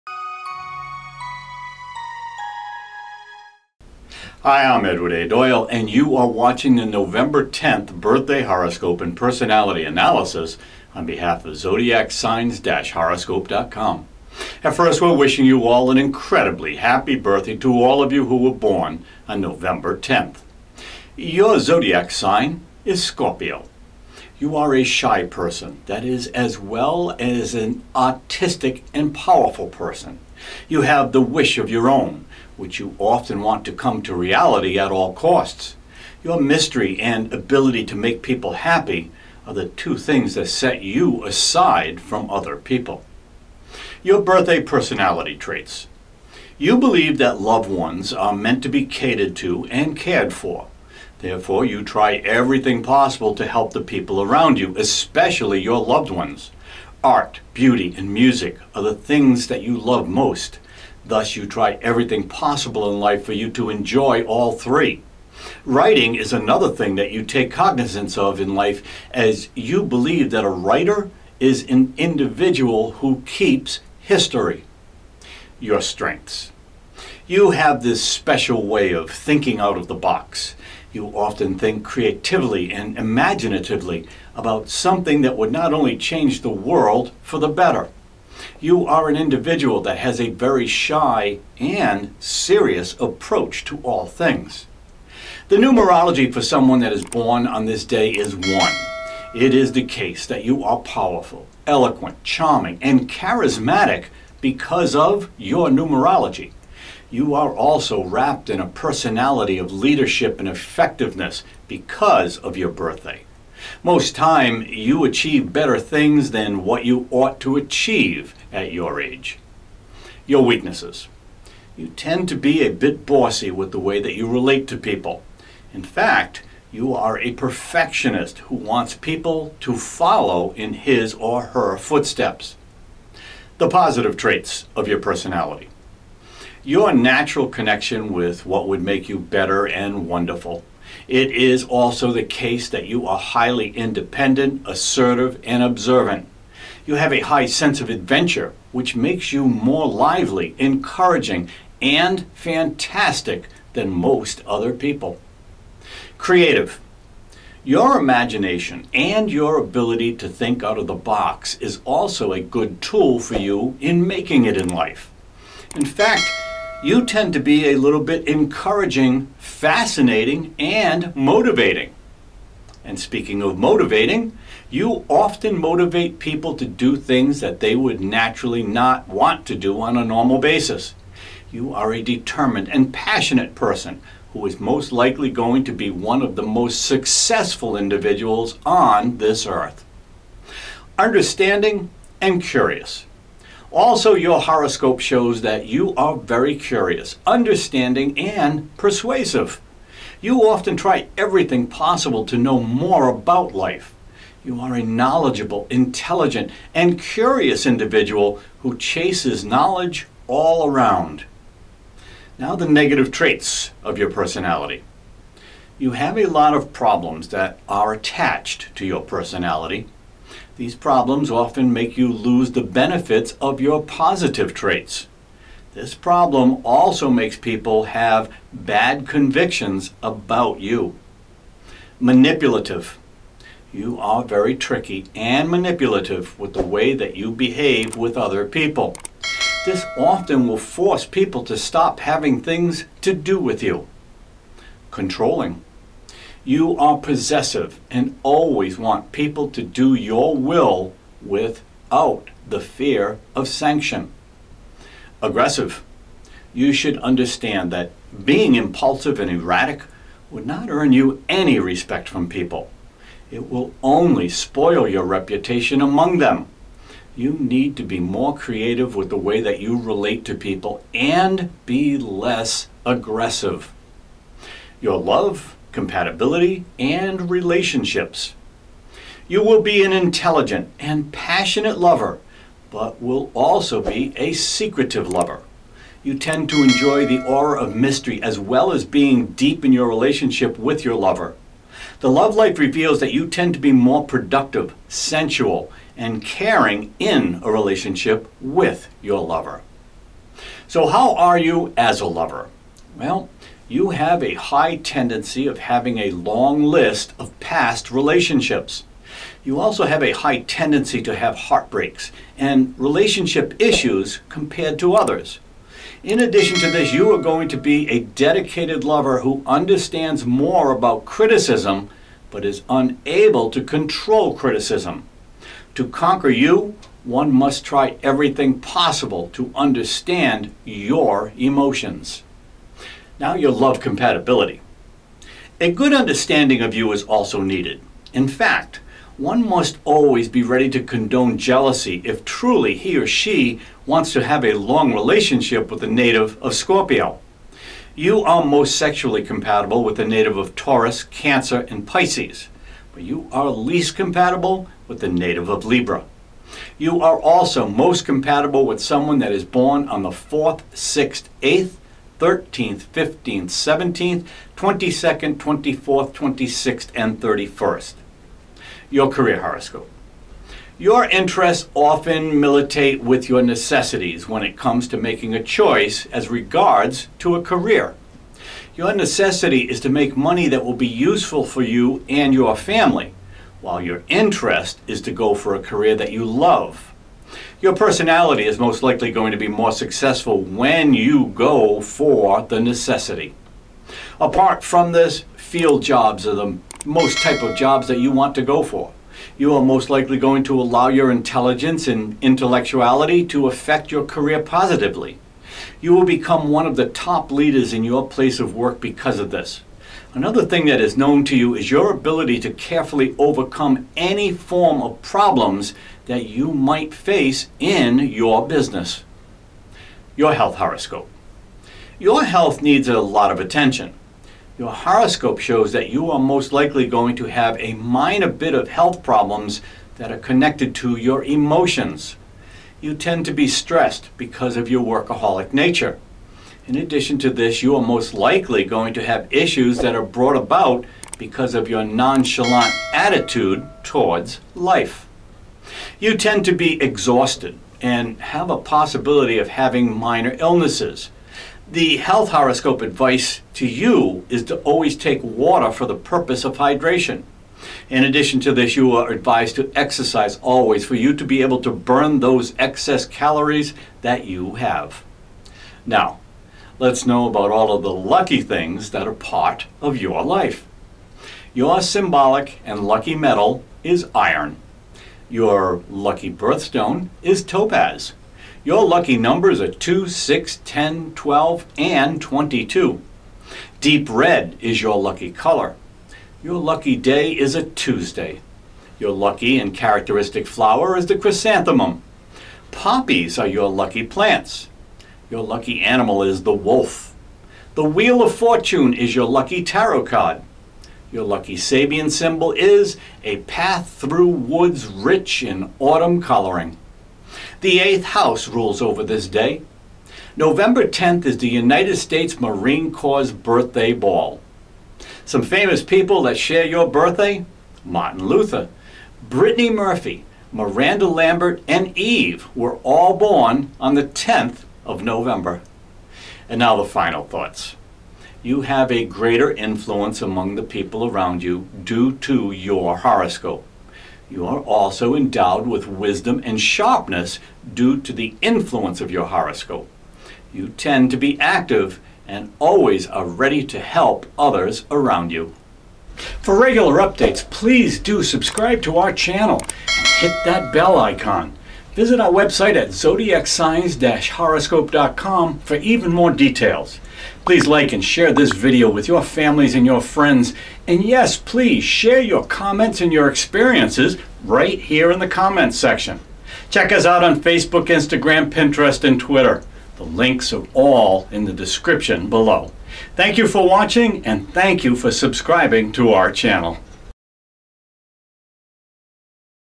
November 10 Zodiac Horoscope and Birthday Personality | November 10th Birthday Personality Analysis
November 10th Birthday Astrology, Your Love, Health, and Career Horoscope

November 10th Zodiac Sign is Scorpio. You have this special way of thinking out of the box. You often think creatively and imaginatively about something that would not only change the world for the better. You are an individual that has a timid and serious approach to things.

Famous People Born on November 10th

Why do Successful People want to know their Birthday Horoscope?

Host: Edward A Doyle

For any suggestions and queries, please make comments below.

A Beginner's Guide To November 10th Birthday Astrology Predictions

00:00 - Introduction
00:43 - November 10th Birthday Personality Traits
01:15 - Strengths
02:00 - Weaknesses
02:13 - Positive Traits Of Your Personality
03:32 - Negative Traits Of Your Personality
04:34 - Love, Compatibility, And Relationships
05:00 - How Are You As A Lover?
05:32 - Love Compatibility
06:08 - November 10th Career Horoscope
06:59 - Health Horoscope
07:46 - Know About Your Lucky Things
08:46 - The Final Thoughts